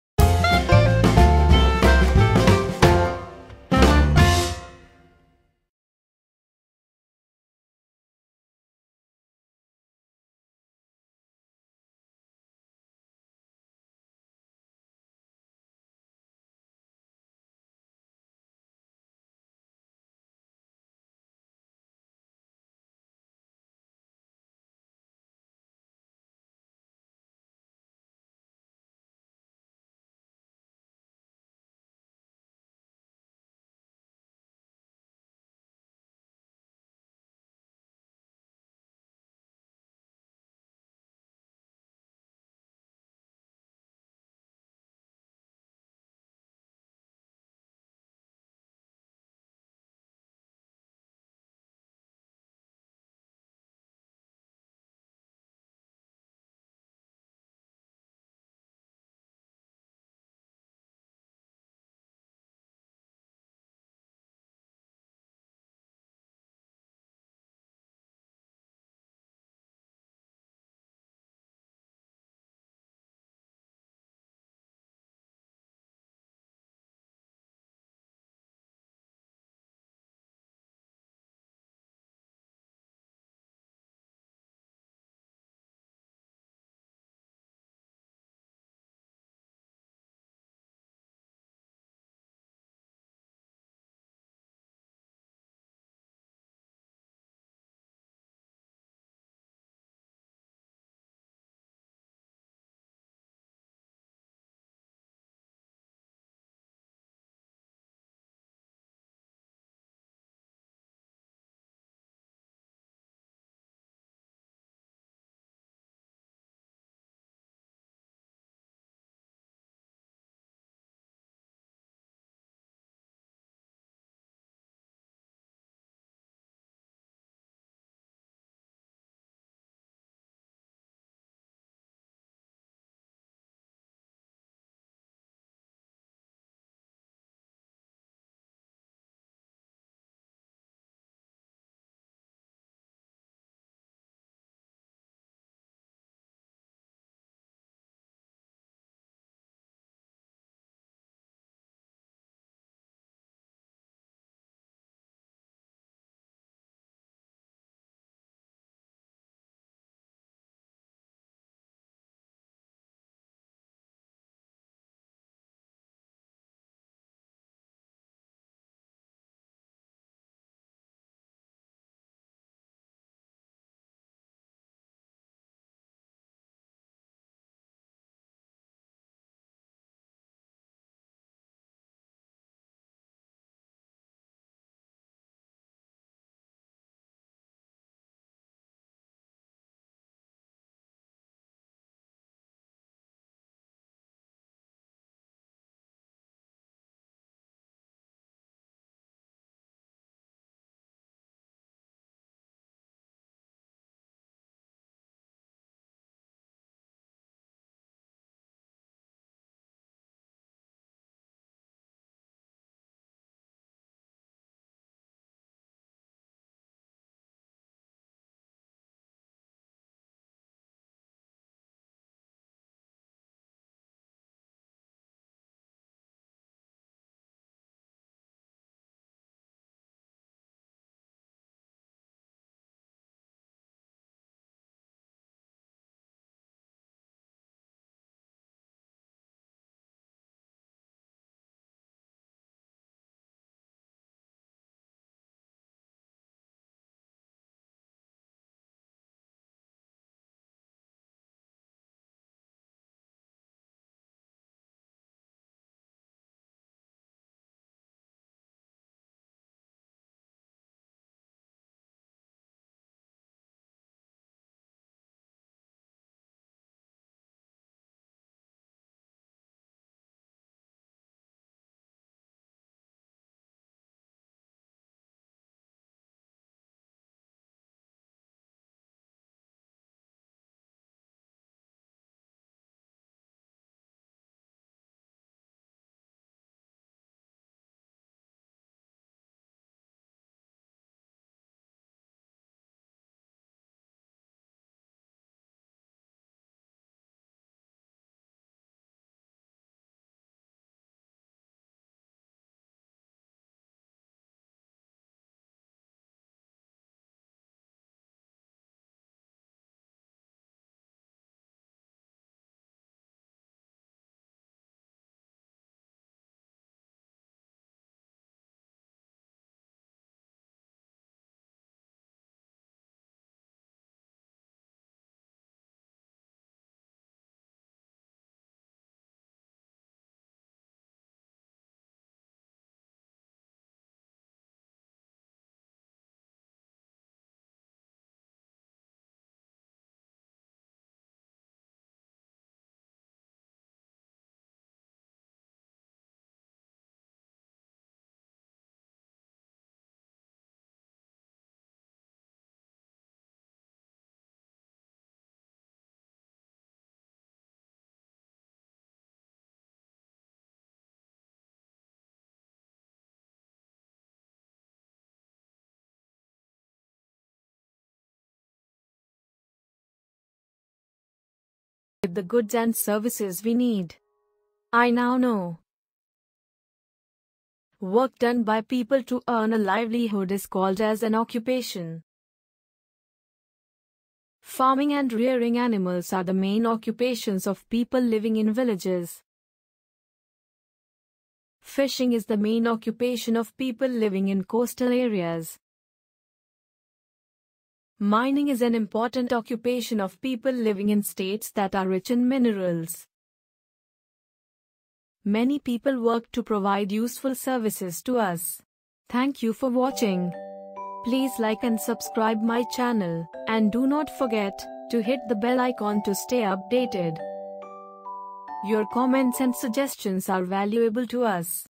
Our Occupations~ Class-3 Social Studies - CBSE / NCERT Syllabus
Learn the lesson " Our Occupations" of class 3 social studies.